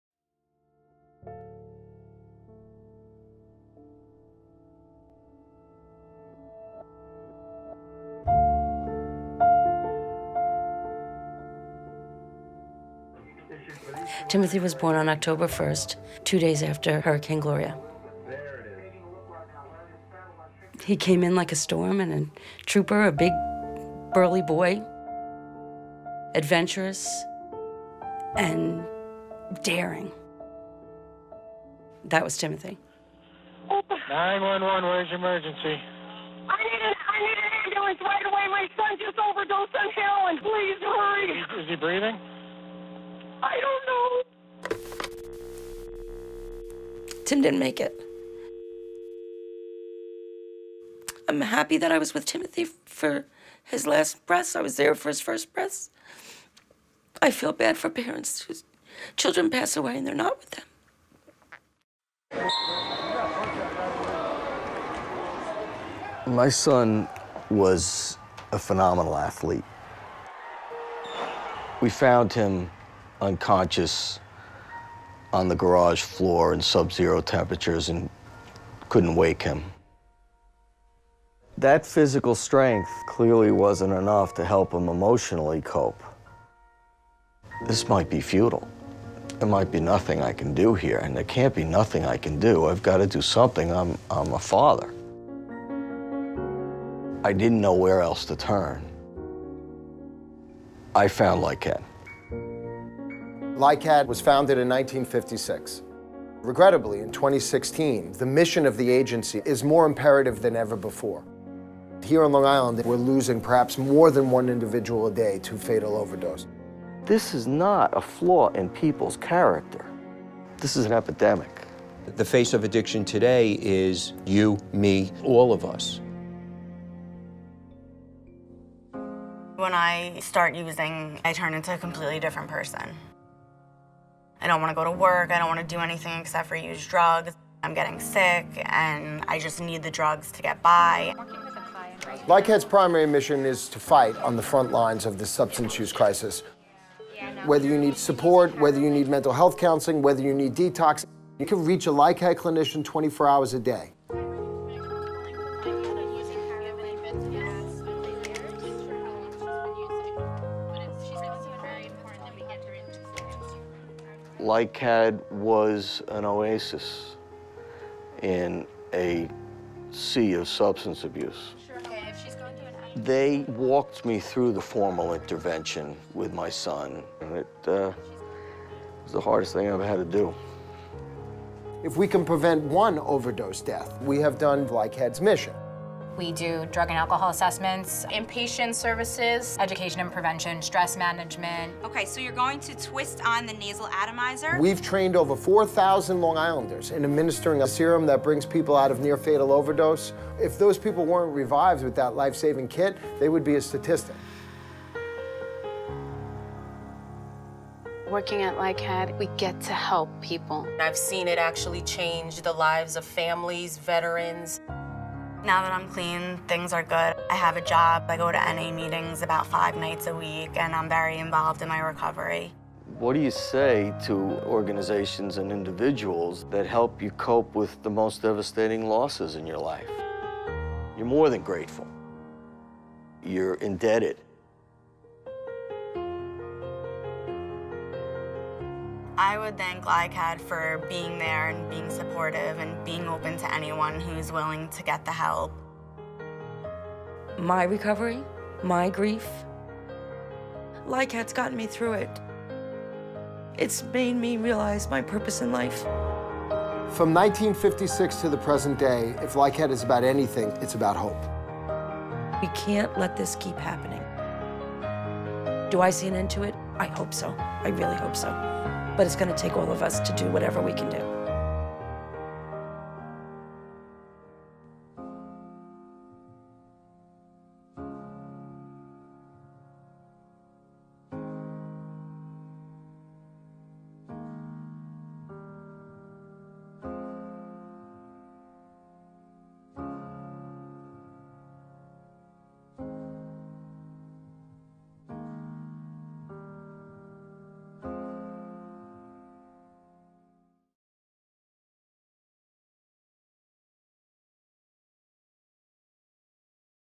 LICADD - 60 Years of Service
This film is dedicated to the thousands of individuals who have volunteered their time, compassion and resources to forward the mission of LICADD from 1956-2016. We thank you.